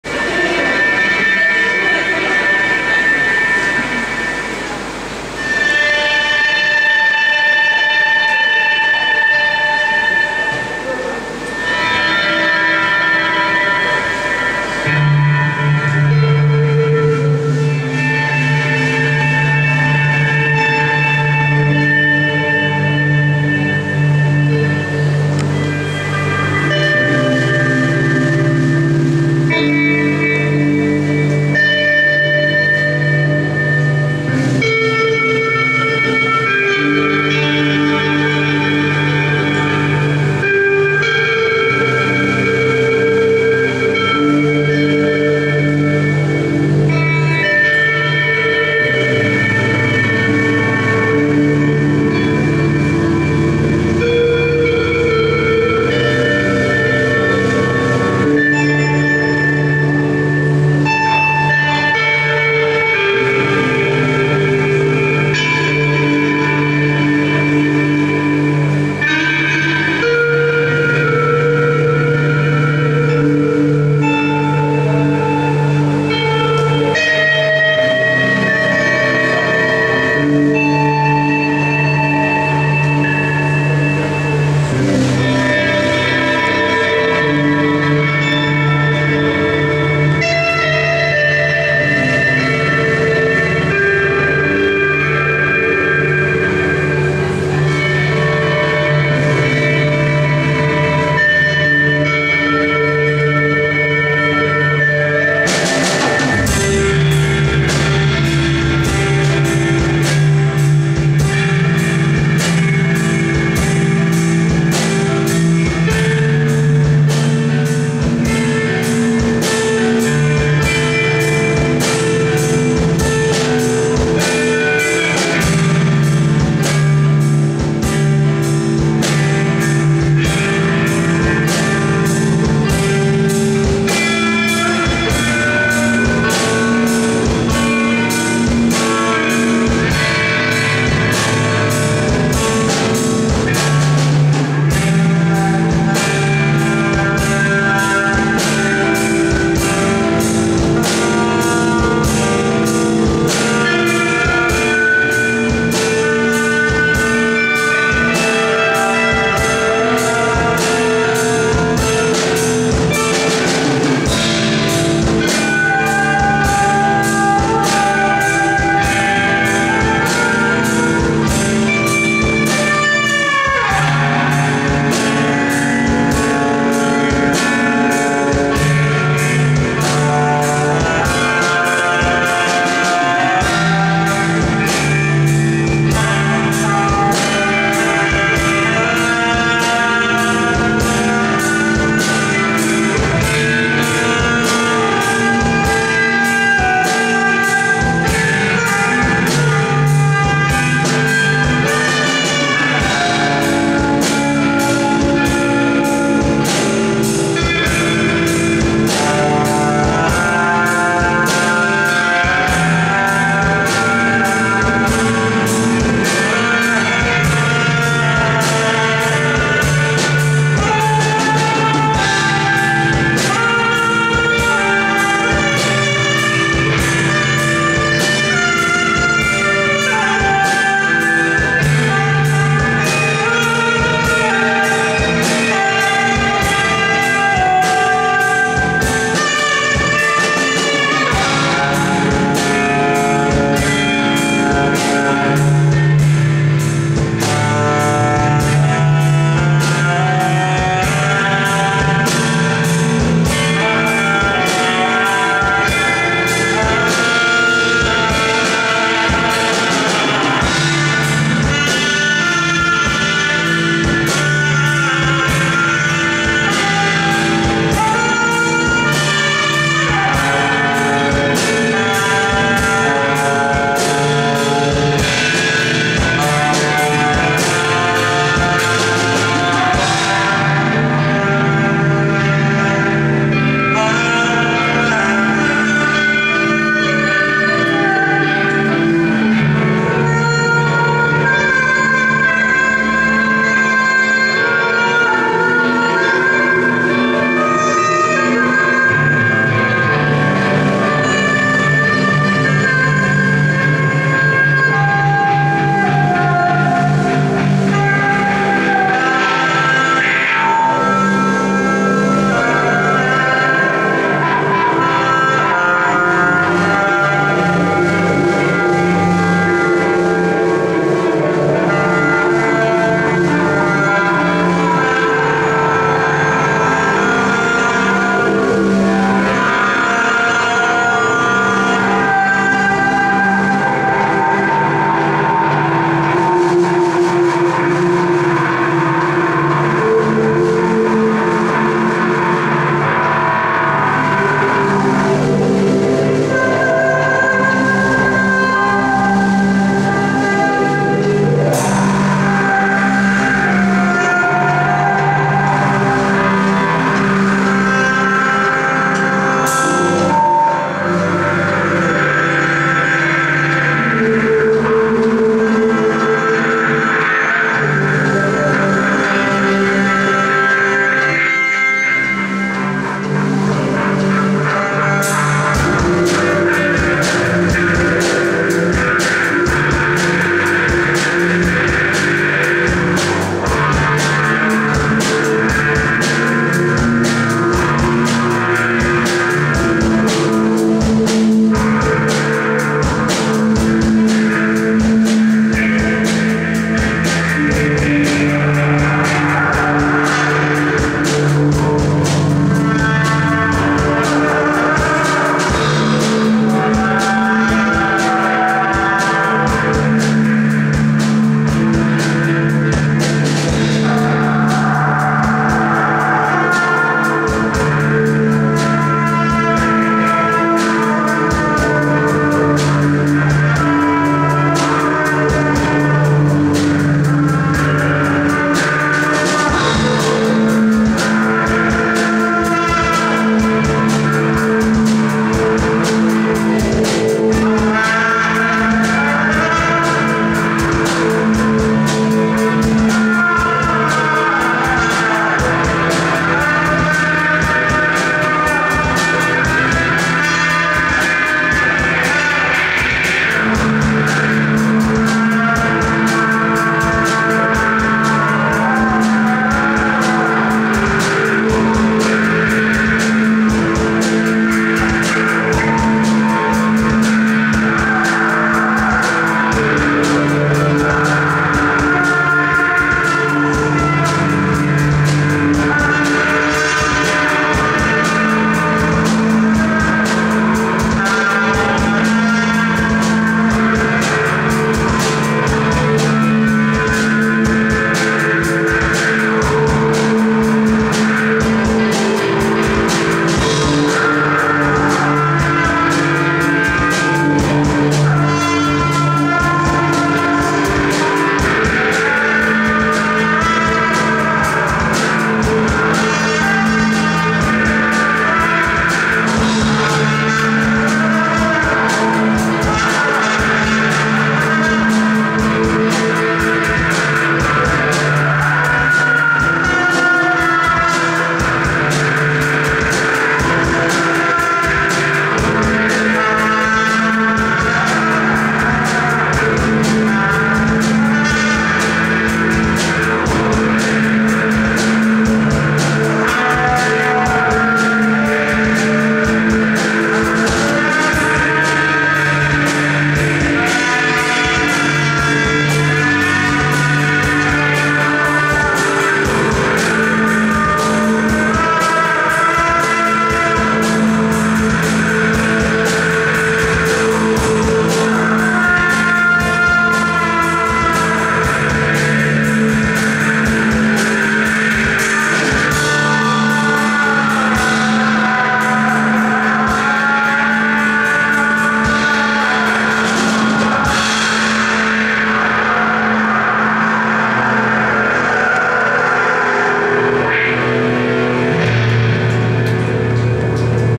WRIST - #002 (2011-07-31)
31 июля 2011 года московская команда WRIST выступила на тонально-ритмической сцене Арт-Этажа «Шоколадная фабрика» на «Alt+E Fest PARTY».

Состав: Алексей Гусаков (барабаны), Антон Ефимов (гитара), Алексей Петраков (гитара, диджериду, эффекты (mini kaoss pad)), Евгения Сивкова (саксофон, труба), Дмитрий Чечеватов (бас).